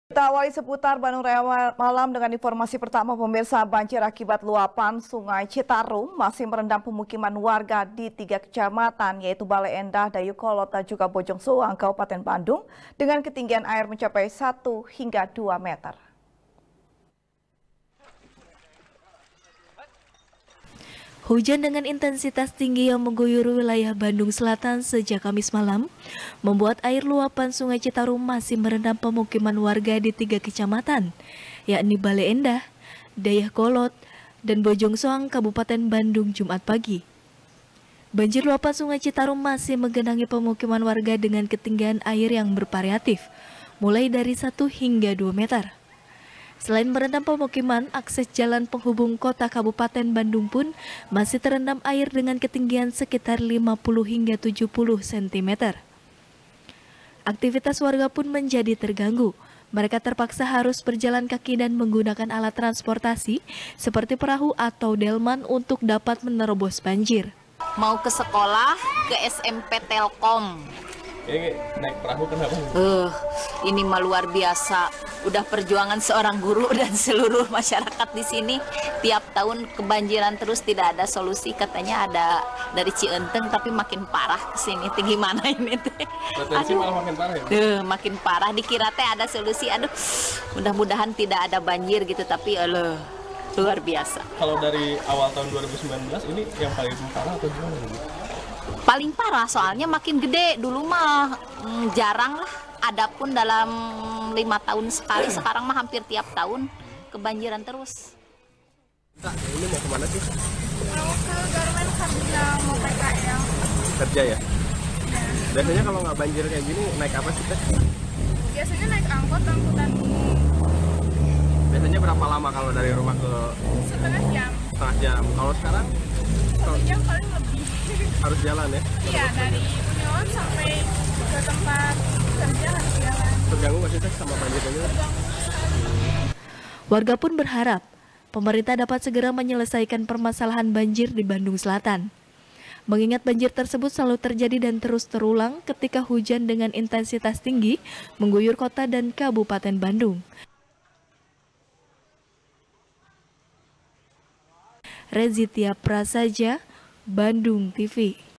Banjir Masih Merendam Pemukiman Warga Di Tiga Kecamatan | SBR | BANDUNGTV
SBR | BANDUNGTV | FARHAT MUMTAZ | Banjir Akibat Luapan Sungai Citarum, Masih Merendam Pemukiman Warga, Di Tiga Kecamatan Yakni Baleendah, Dayeuh Kolot, Dan Bojong Soang, Kabupaten Bandung, Dengan Ketinggian Air, Mencapai Satu Hingga Dua Meter.